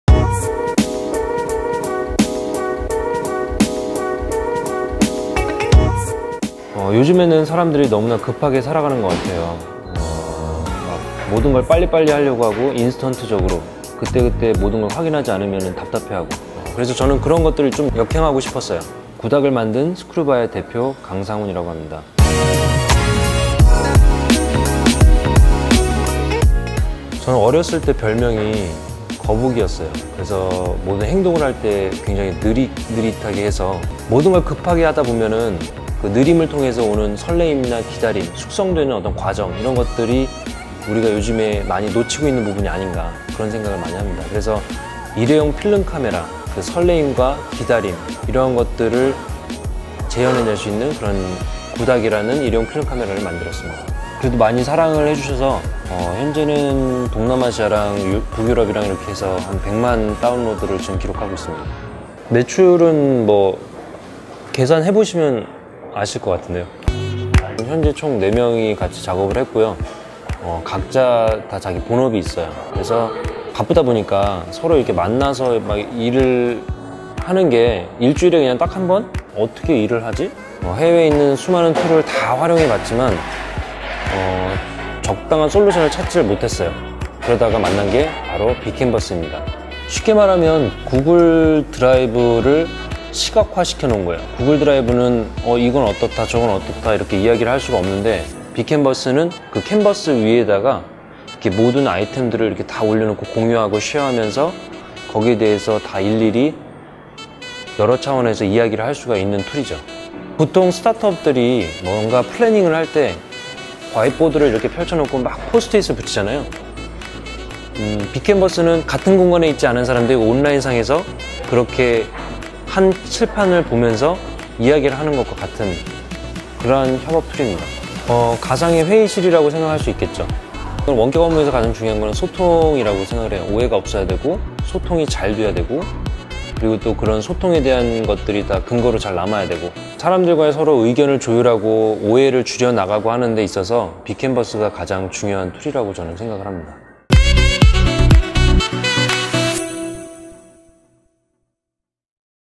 구닥(Gudak) X 비캔버스(BeeCanvas)_스타트업스 코리아
스타트업스 코리아

스타트업 '스크루바'의 일회용 필름 카메라앱 '구닥'은 출시 3개월 만에 애플 아이폰 앱스토어에서 100만 다운로드 돌파! 국내에선 7월 중순 이후 줄곧 다운로드 1위를 달리고 있으며, 해외 17개 국가에서도 1위 기록!
스크루바 팀원들은 모두 N잡러(여러 개의 일자리를 가진 사람)이다. 서로 바쁜 나머지 철저하게 '원격업무'로 '구닥(Gudak)'을 만들어 낼 수 있었던 '협업방식'은 무엇이었을까?